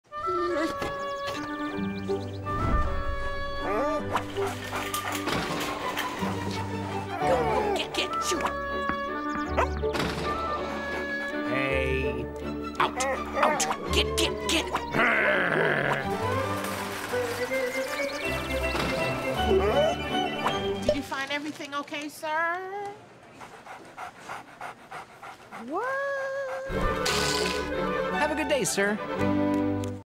Doritos Dogs - Crash the Super Bowl 2016 WINNER OFFICIAL
Doritos Crash the Super Bowl 2016 WINNER. Starring Miz, Bolt, and Logan - three very talented rescue dogs. Director/Writer: Jacob Chase, Producer/Writer: Travis Braun

Director of Photography: Danny Grunes